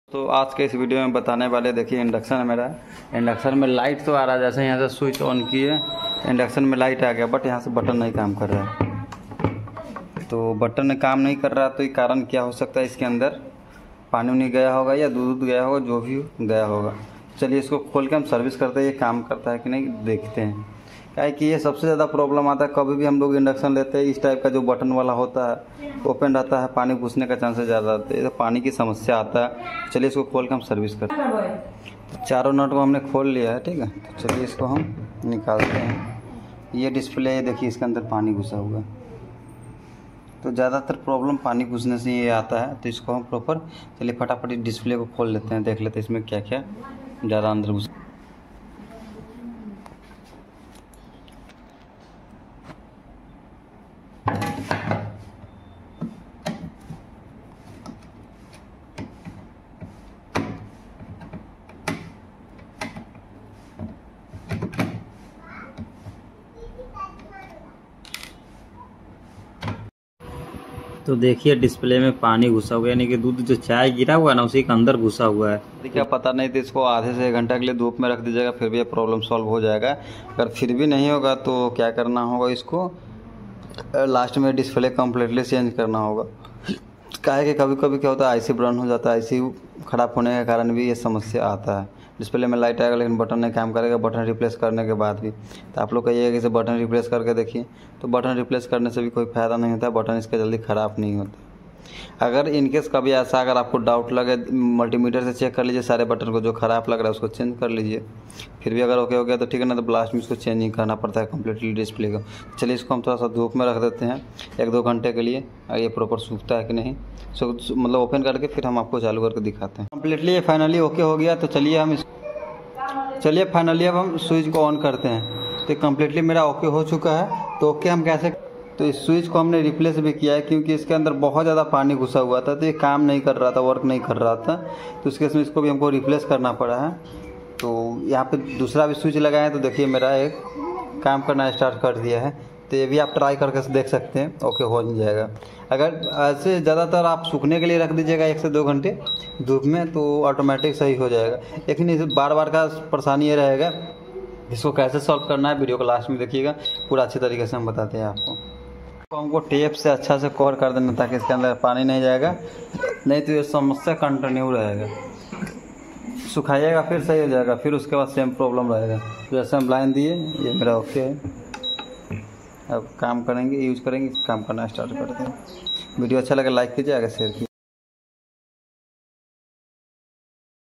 Induction Me Light Aata Lekin Power Wala Button Kaam Nahi Kar Raha Hai || induction cooker kaise
Induction Me Light Aata Lekin Power Wala Button Kaam Nahi Kar Raha Hai ||